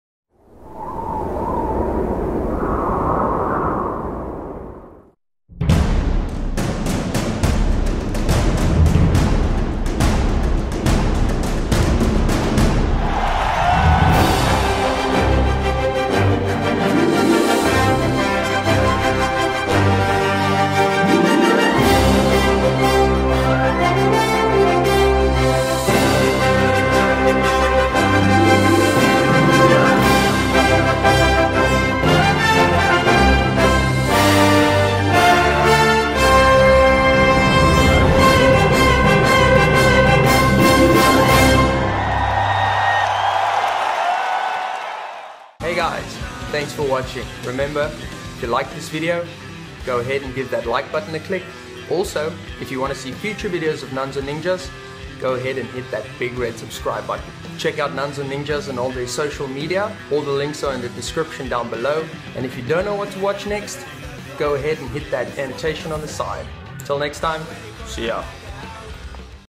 The Rabbit Hole Teaser Trailer
Nuns Or Ninjas Live at The Rabbit hole in Durbanville Teaser Trailer

27 Nov 2015 at 20:00pm

Follow NoN On Social!

For The Best Gear!